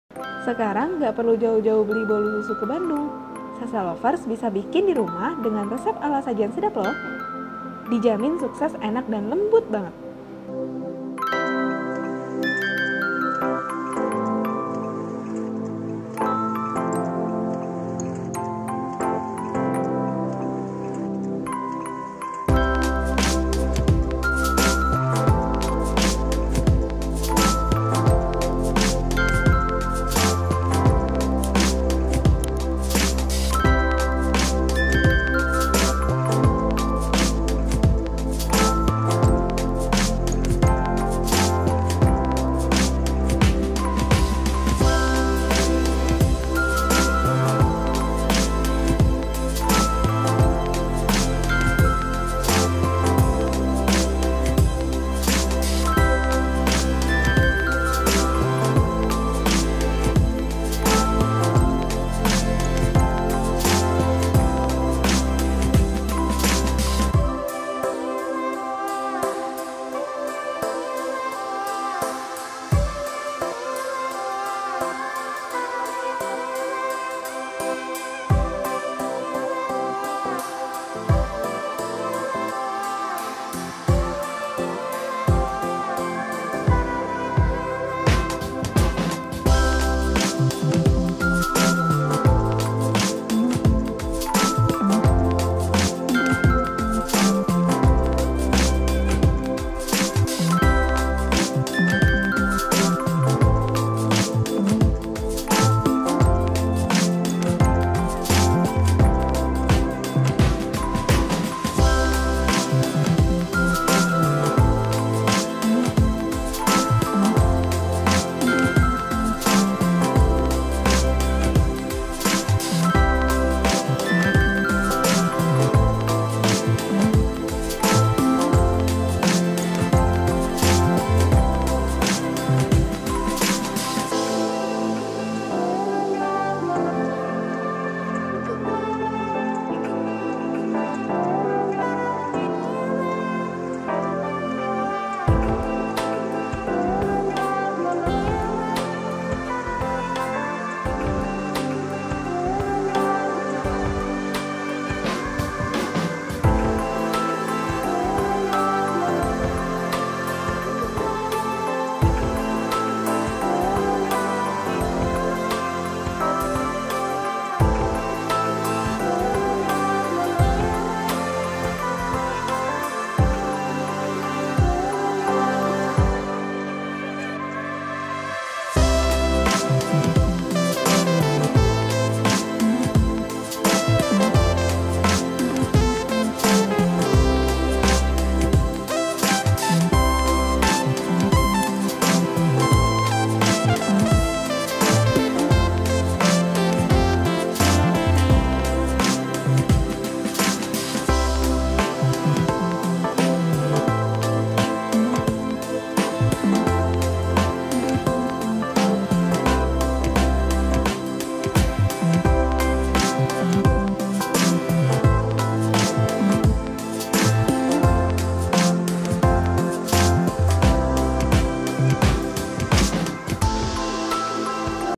IDE JUALAN PALING LARIS! INI DIA CARA MEMBUAT BOLU SUSU LEMBANG
IDE JUALAN PALING LARIS! INI DIA CARA MEMBUAT BOLU SUSUS LEMBANG

Bahan:
4 btr telur
½ sdt emulsifier
120 gr gula pasir
160 gr tepung terigu
15 gr tepung maizena
70 ml minyak
100 ml susu cair

Bahan taburan:
100 gr krim bubuk, kocok dengan 200 ml air dingin
100 gr keju cheddar, parut panjang

Produser Eksekutif: Intan Yusan Septiani
Penulis Resep: Indri, Resi, Qorin, Ruth
Penulis Naskah: Zahra
Peraga: Resi, Indri, Qorin, Ruth
Fotografer: Andrian, Osyad, Bram, Yudi
Videografer: Andrian, Osyad, Bram, Yudi
Video Editor: Andrian, Osyad, Bram, Yudi

IDE JUALAN PALING LARIS! INI DIA CARAA MEMBUAT BOLU SUSU LEMBANG